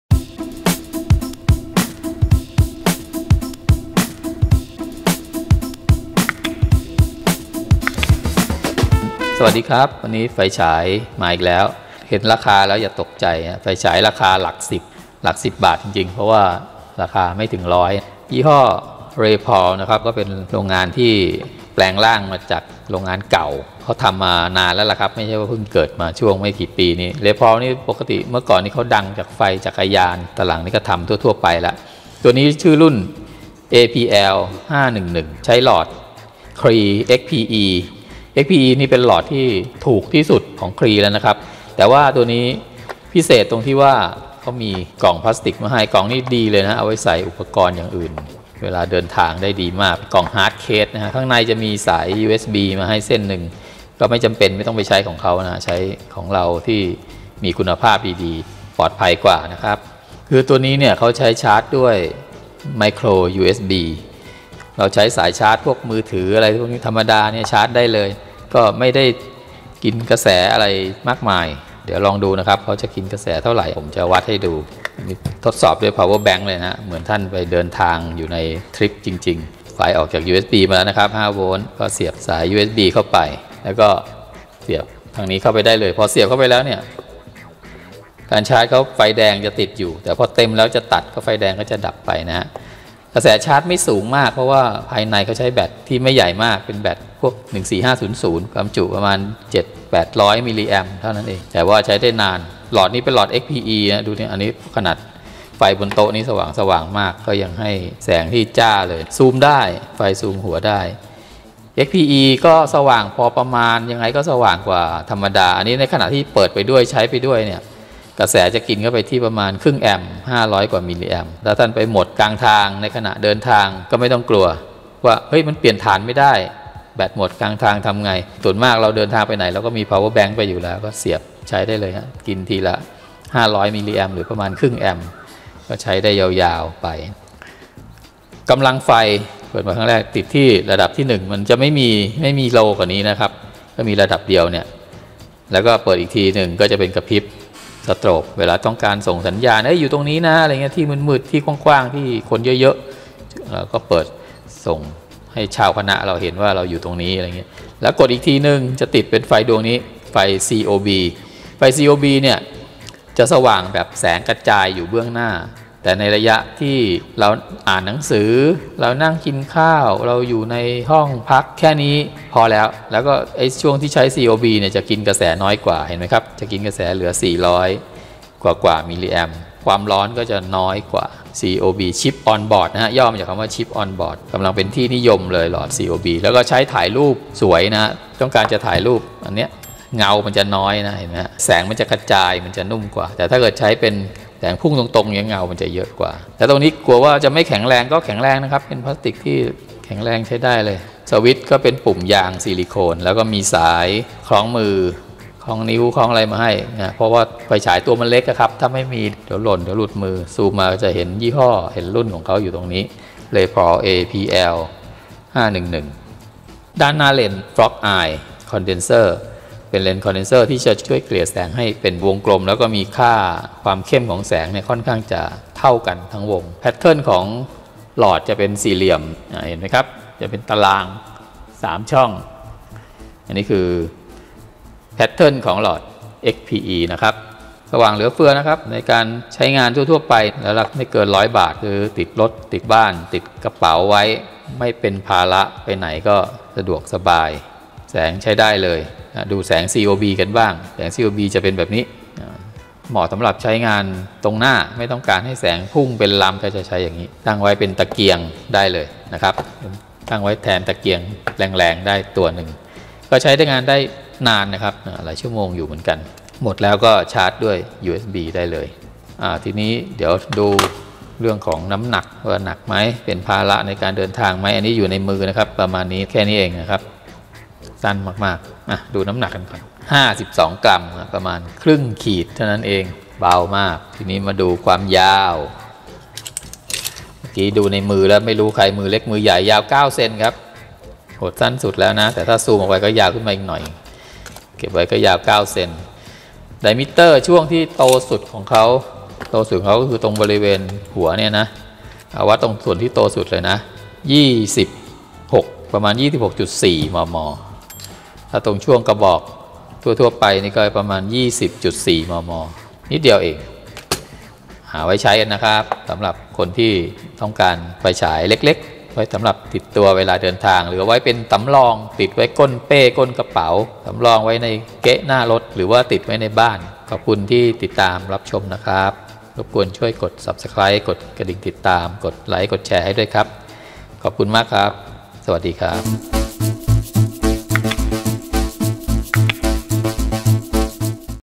Easy Travel : Raypal APL-511 ไฟหลายราคาไม่กี่สิบ ติดกระเป๋าไว้ ไม่ต้องเสียดาย
ไฟฉายเล็กๆ เบาๆ มีแบตฯลิเธียมในตัว
เปิดต่อเนื่องได้นาน3-4ชม. เป็นอย่างน้อย
ชาร์จด้วย USB หรือ Power bank
หลอดหน้า CREE XP-E **ถ้าเจอของแท้นะ
หลอดข้าง COB
คุณสมบัติแค่นี้ก็เหลือ สำหรับติดกระเป๋าตลอดการเดินทาง
หาของในที่มืดๆ บนรถ บนเครื่องบิน
เข้าชมสถานที่ต่างๆที่แสงน้อย
ส่งสัญญาณระบุตำแหน่งกับเพื่อนร่วมทริป
ออกแต่เช้ามืด หรือกลับค่ำ ได้ใช้แน่นอน

ราคาไม่กี่สิบ ติดไว้ไม่เสียหาย
ถึงจะมีไฟฉายดีๆแรงๆอยู่แล้ว แต่มีไว้สำรอง หรือแบ่งให้เพื่อนร่วมทริปถือไว้อีกอัน ปลอดภัย สบายใจกว่า